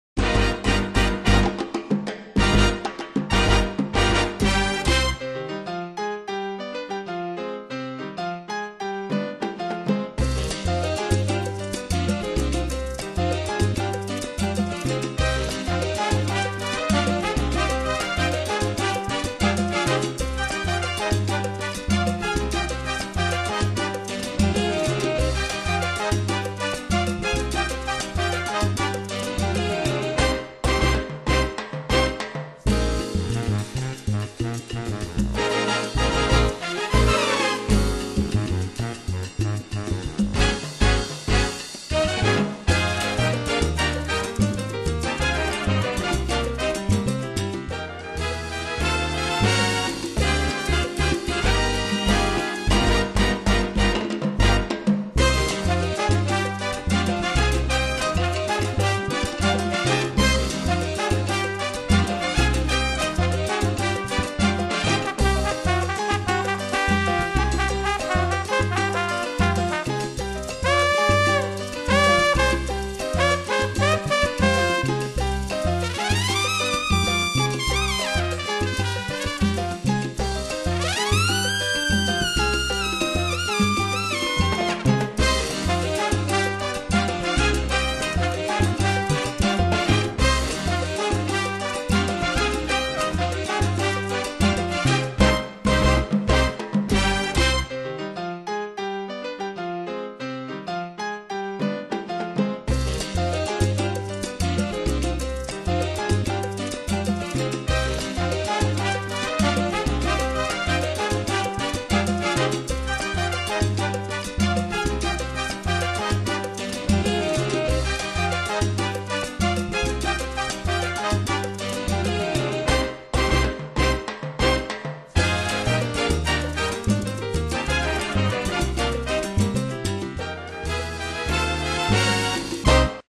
El pallasso malabarista Pauillo
Aquí em podeu veure fent equilibris amb foc.